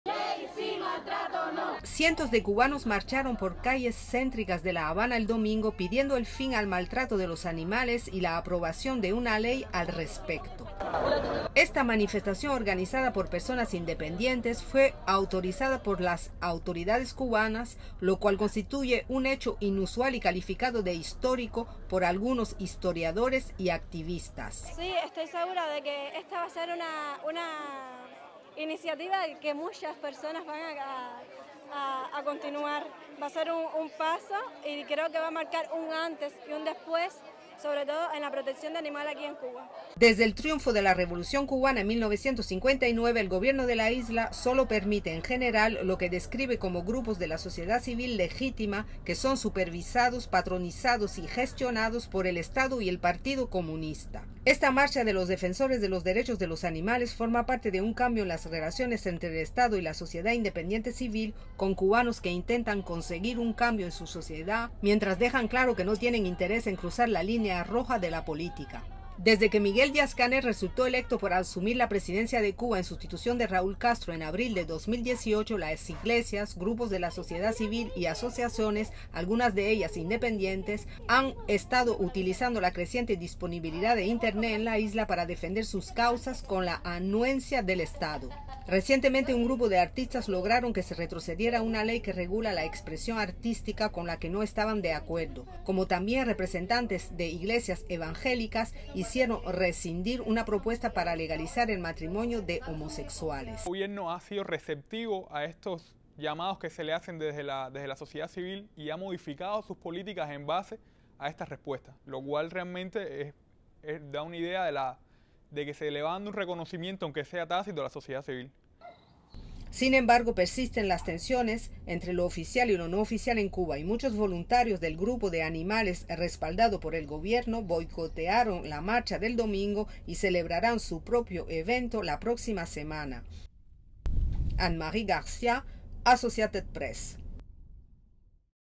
Cuba autoriza marcha de un grupo defensor de animales
(8 Apr 2019) RESTRICTION SUMMARY:
STORYLINE:
Cientos de cubanos marcharon por calles céntricas de La Habana el domingo, pidiendo el fin al maltrato de los animales y la aprobación de una ley al respecto.

Esta manifestación organizada por personas independientes fue autorizada por las autoridades cubanas, lo cual constituye un hecho inusual y calificado de histórico por algunos historiadores y activistas.

Soundbite:  Beatriz Del Carmen Hidalgo, Organizadora de la marcha:
"Sí estoy segura que esta va a ser una iniciativa en que muchas personas van a continuar, va a ser un paso y creo que va a marcar un antes y un después sobre todo en la protección de animales aquí en Cuba".

Desde el triunfo de la Revolución cubana en 1959, el gobierno de la isla  sólo permite, en general, lo que describe como grupos de la "sociedad civil legítima" que son supervisados, patrocinados y gestionados por el Estado y el Partido Comunista.

Esta marcha de los defensores de los derechos de los animales forma parte de un cambio en las relaciones entre el Estado y la sociedad independiente civil, con cubanos que intentan conseguir un cambio en su sociedad mientras dejan claro que no tienen interés en cruzar la línea roja de la política.

Desde que Miguel Díaz-Canel resultó electo para asumir la presidencia de Cuba en sustitución de Raúl Castro, en abril de 2018, las iglesias,
RESTRICTION SUMMARY: AP CLIENTS ONLY
ASSOCIATED PRESS – AP CLIENTS ONLY
La Habana, Cuba – 5 Abril 2019
grupos de la sociedad civil y asociaciones, algunas de ellas independientes, han estado utilizando la creciente disponibilidad de Internet en la isla para defender sus causas con la anuencia del Estado.

Recientemente un grupo de artistas lograron que se retrocediera una ley que regula la expresión artística con la que no estaban de acuerdo, como también representantes de iglesias evangélicas hicieron rescindir una propuesta para legalizar el matrimonio de homosexuales.

En tanto se organizó ayuda independiente para las víctimas de un tornado que azotó La Habana en enero.

Soundbite: Jayssel Padron, Bloguero:
"El nuevo gobierno ha sido receptivo a estos llamados que se le hacen desde la Sociedad civil y ha modificado sus políticas en base a esta respuesta, lo que da una idea de que se comienza un reconocimiento, aunque sea tácito, a la Sociedad civil", dijo este bloguero.

Sin embargo, persisten las tensiones entre lo oficial y lo no oficial en Cuba, y muchos voluntarios del grupo de animales respaldado por el gobierno boicotearon la marcha del domingo y celebrarán su propio evento la próxima semana.